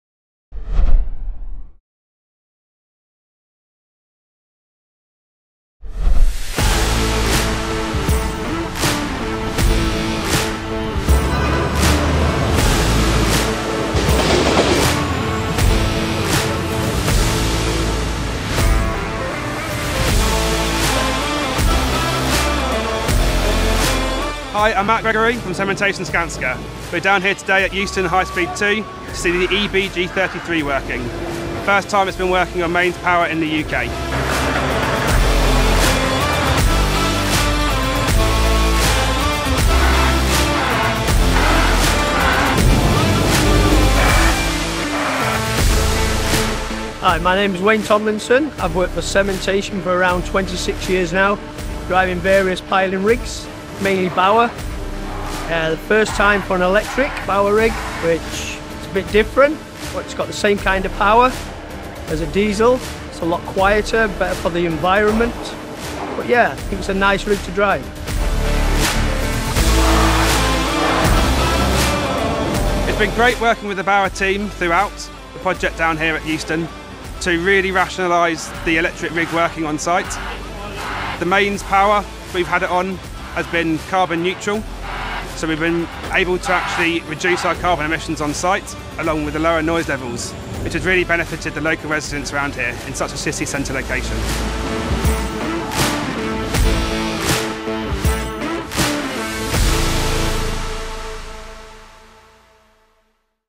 BAUER Maschinen GmbH – Elektrische eBG 33 überzeugt in London
Volle elektrische Power voraus. Unsere eBG 33 überzeugt am HS2 Projekt in Euston, London. Im Video erfahrt ihr, warum dem Gerätefahrer die Arbeit mit unserem elektrischen Bohrgerät besonders viel Spaß bereitet.